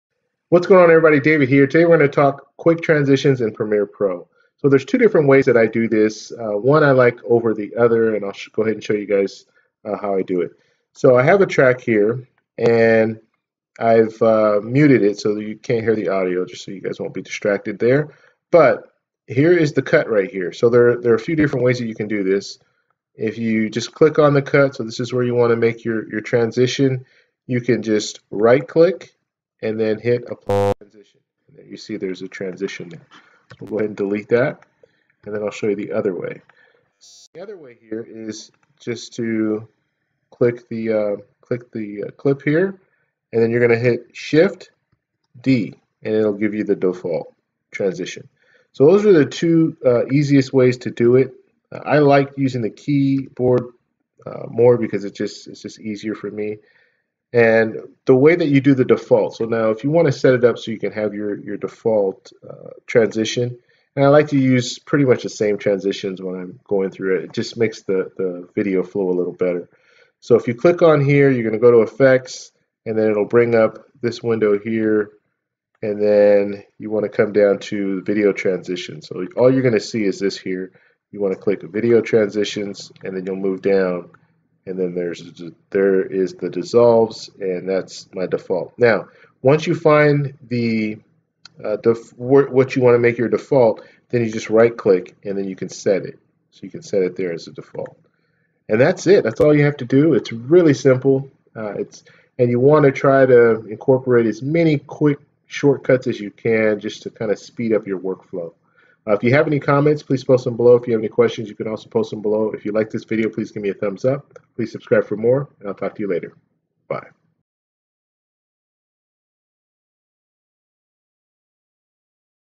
Transition Shortcuts in Premiere Pro CC
Find out how to expedite your video editing process by using Premiere Pro shortcuts.